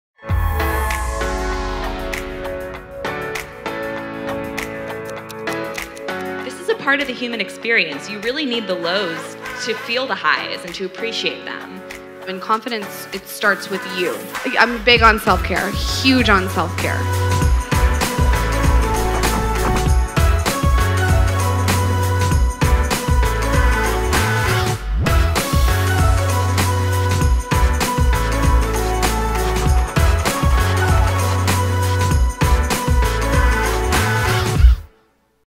ShelfTalk Conference hosted by NeighborhoodGoods x Create & Cultivate
Create & Cultivate teamed up with Neighborhood Goods to host Shelf Talk: a conversation around beauty and wellness. Over 250 attendees came to hear from speakers like Lauryn Evarts Bosstick, Ashley Robertson, Candace Hampton, Courtney Newell, and more!